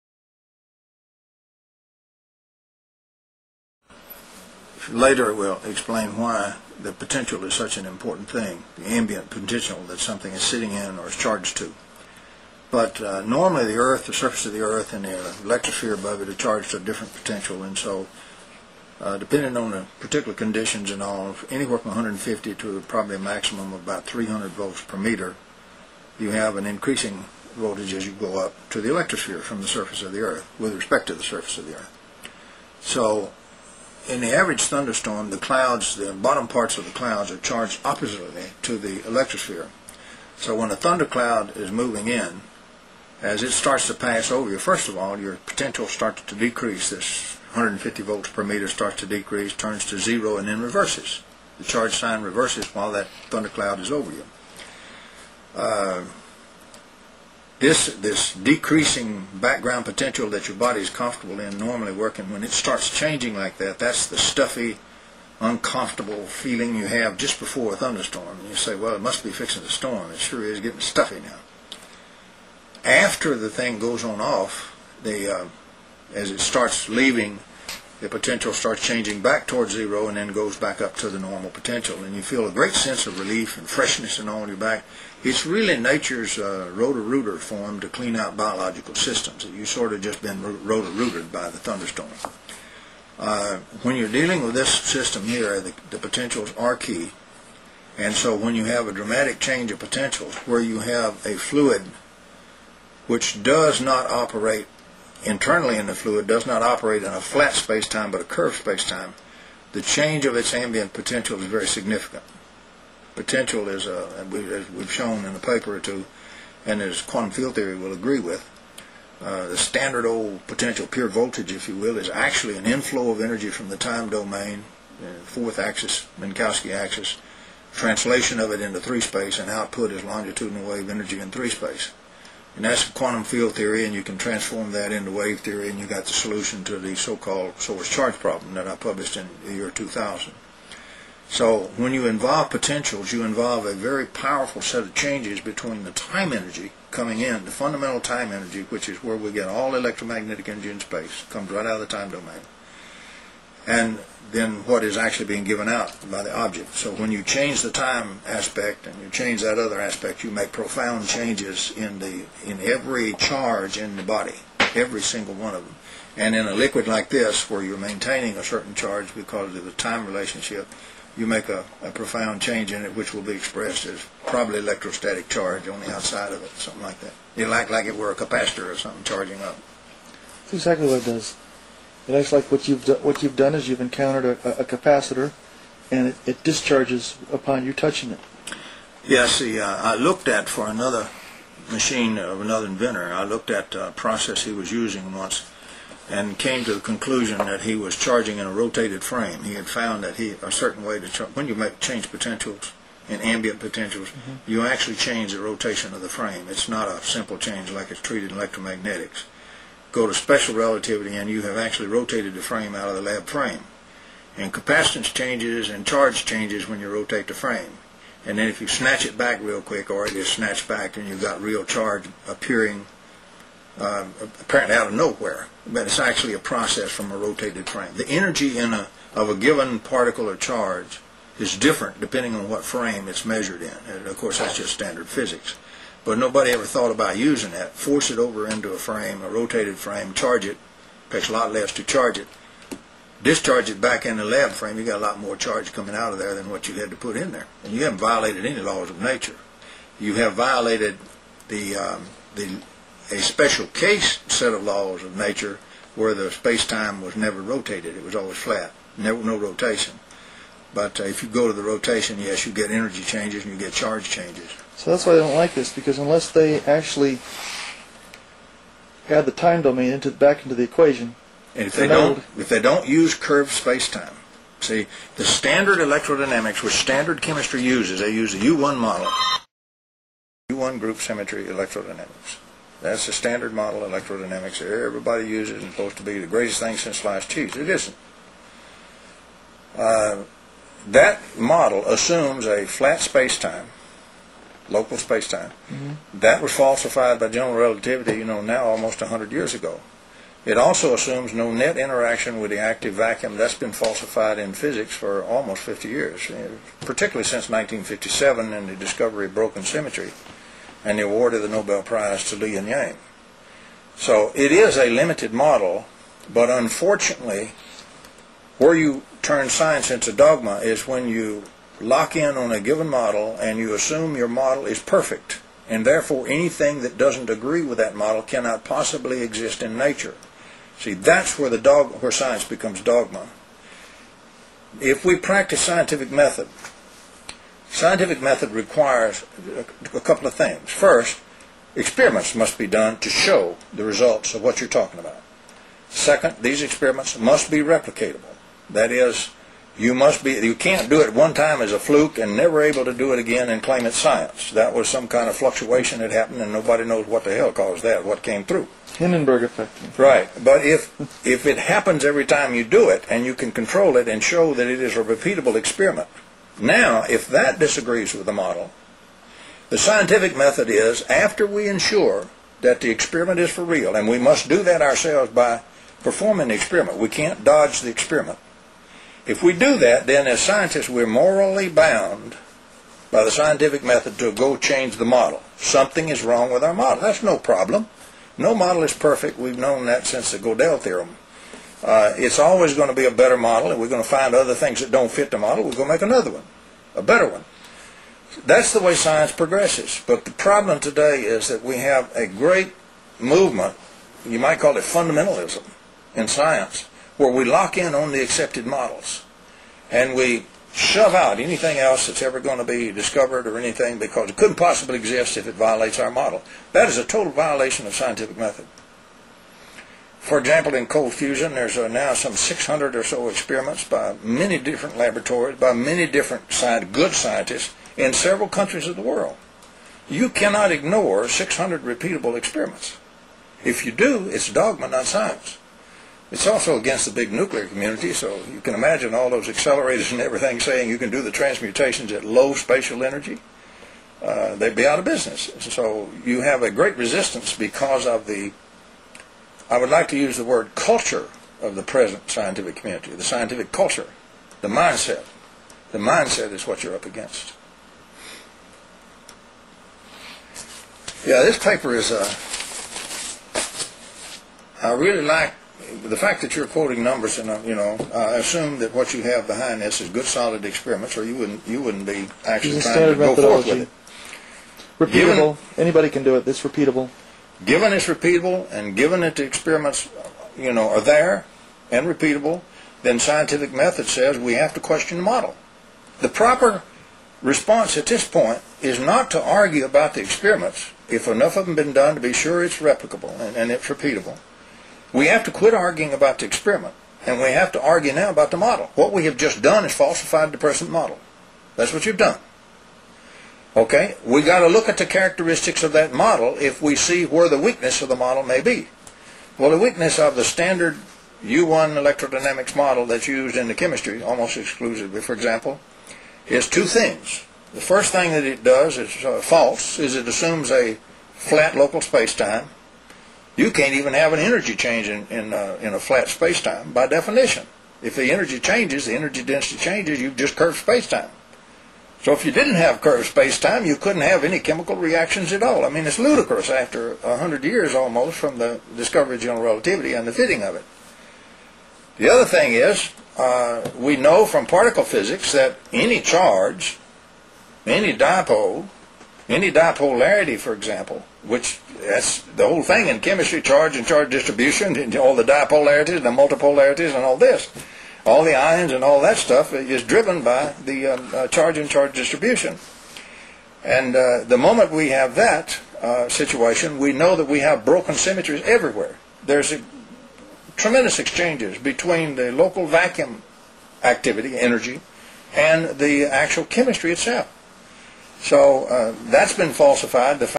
Dr. Tom Bearden: Interview on H3O / HRx / Scalar Wave Technology 1/8
Dr. Tom Bearden is interviewed by Greg Caton (Nov., 2002) at Bearden's home in Huntsville, Alabama  concerning the properties of H3O / HRx (products carried by Alpha Omega Labs) and the brave new world of applied scalar wave technology.  Out of the theories discussed in this interview, AO Labs went on to produce its own manufacturing plant in Ecuador, making these solutions with additional functional properties not found in the extant literature (2009).

For more information about H3O, its anti-aging and wound healing properties, and the direction that further research in South America is taking, see :